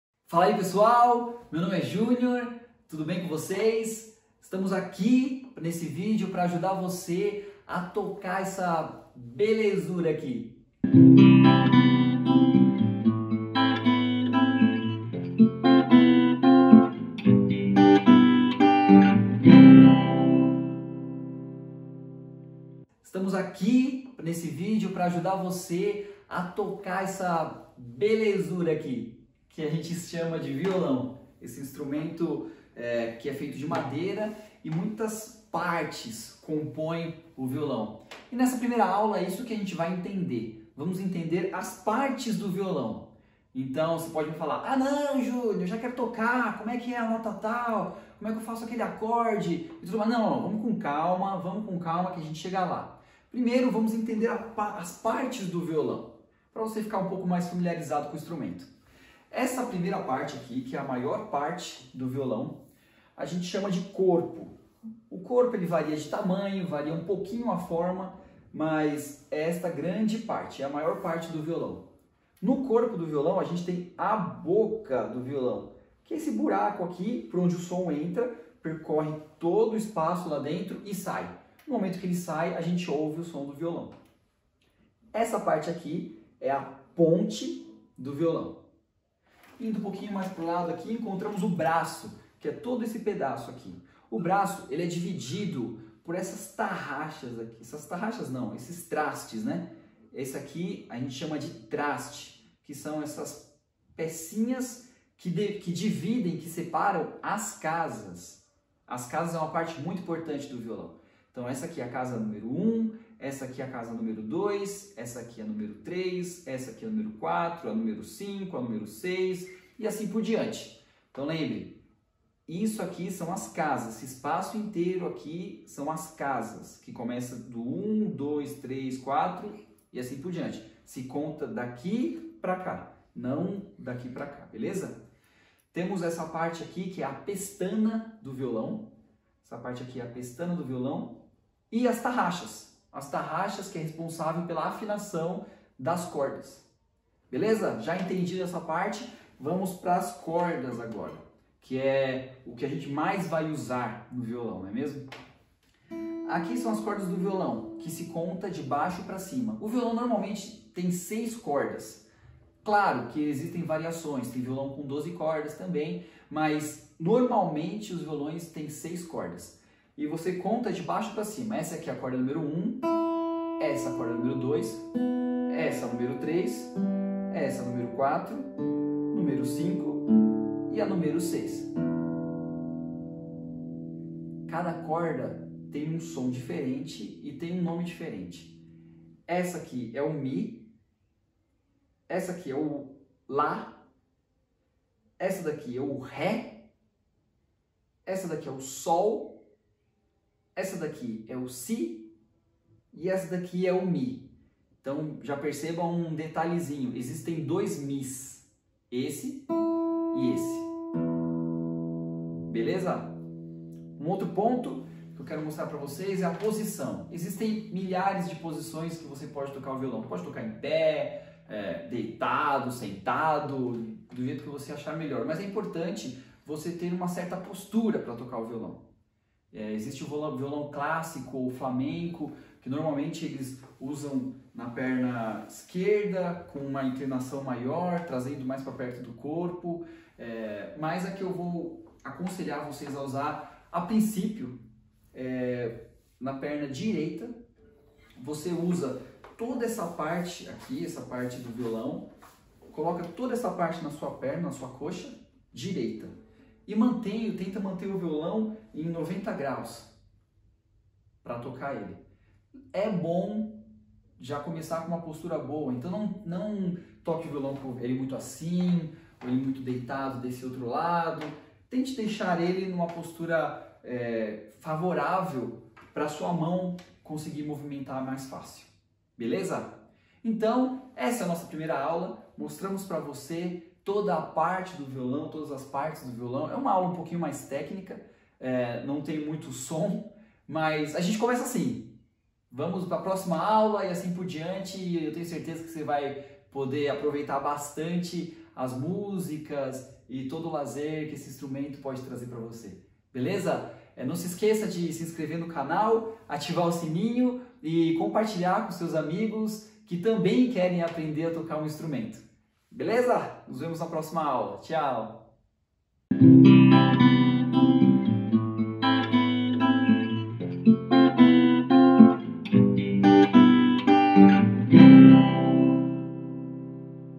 01 - Aula de violão (para iniciante)
Aprenda a tocar violão com vídeos curtos. Nesta primeira aula de violão é muito importante criar uma base sólida sobre o instrumento. Assim, será mais fácil desenvolver os seus talentos para tocar este instrumento musical.